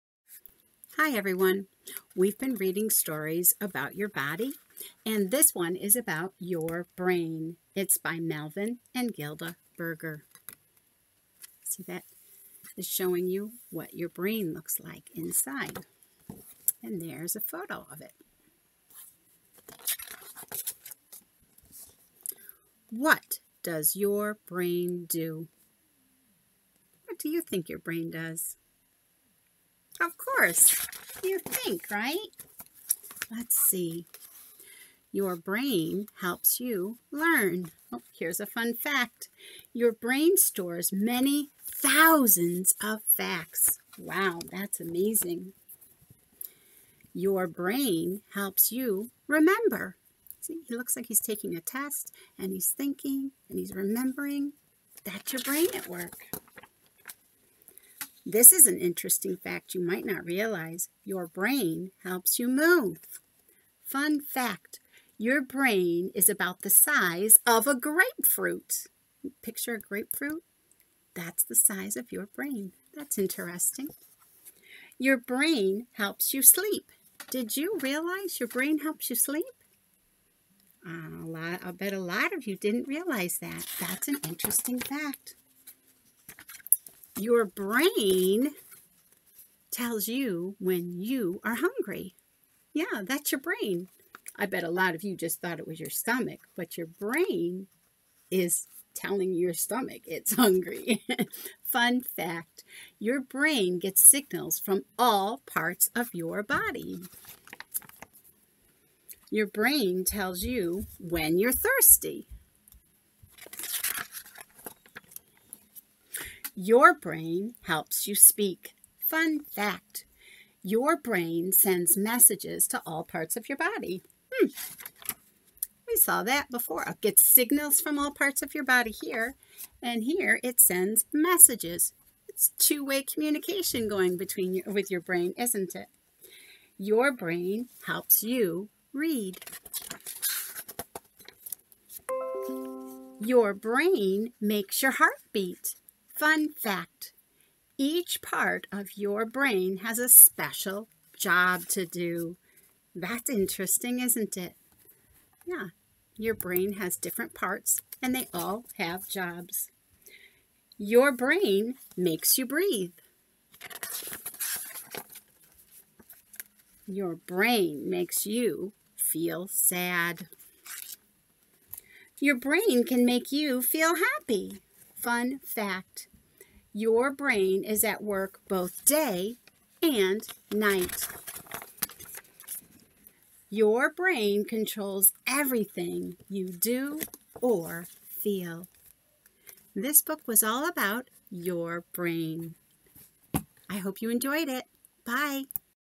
Your Brain
Listen to this factual book to learn about your brain and the amazing things it does for your body.